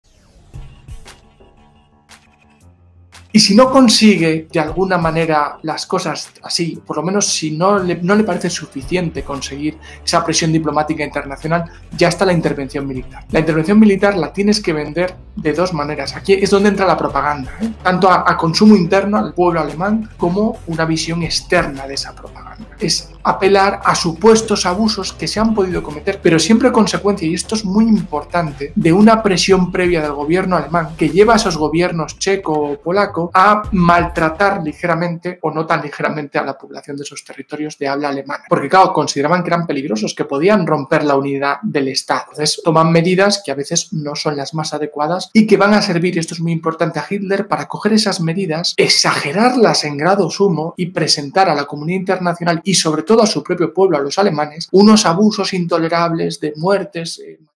🔴 La PROPAGANDA de la ALEMANIA NAZI 📖🙄 Las agresiones de HITLER
[DESCUBRE] cómo la opinión pública de Alemania era manipulada por la propaganda nazi para justificar las agresiones de Hitler y, también, cómo los nacionalsocialistas utilizaban esos mismos mensajes en el ámbito diplomático ✅

Este vídeo es solo un fragmento de uno más extenso sobre la política racial del Tercer Reich. Puedes acceder al [DIRECTO COMPLETO] en el siguiente enlace